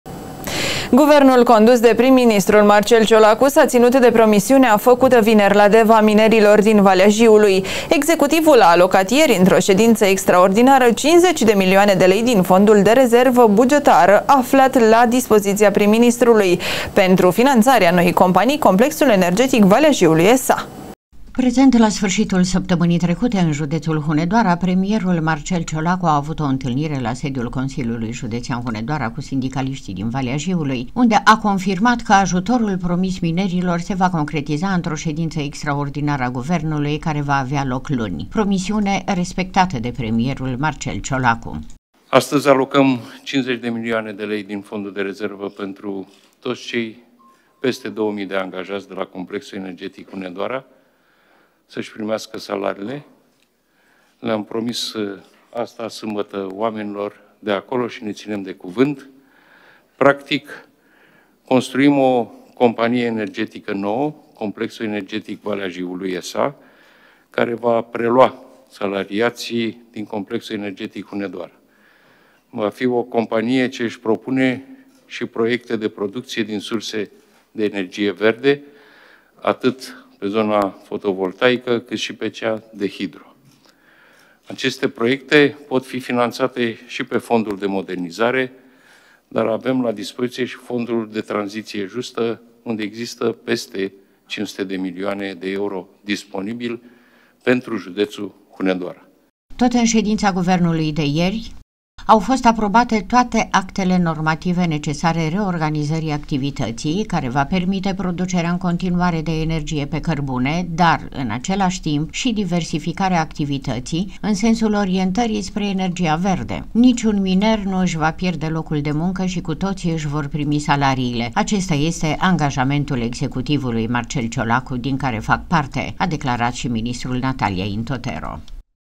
N.INTOTERO: „GUVERNUL CIOLACU ȘI-A RESPECTAT PROMISIUNEA
FĂCUTĂ VINERI, LA DEVA, MINERILOR DIN VALEA JIULUI”
      Guvernul condus de prim-ministrul Marcel Ciolacu s-a ținut de promisiunea făcută vineri, la Deva, minerilor din Valea Jiului. Executivul a alocat ieri, într-o ședință extraordinară, 50 de milioane de lei din Fondul de Rezervă Bugetară aflat la dispoziția Prim-Ministrului pentru finanțarea noii companii Complexul Energetic Valea Jiului S.A.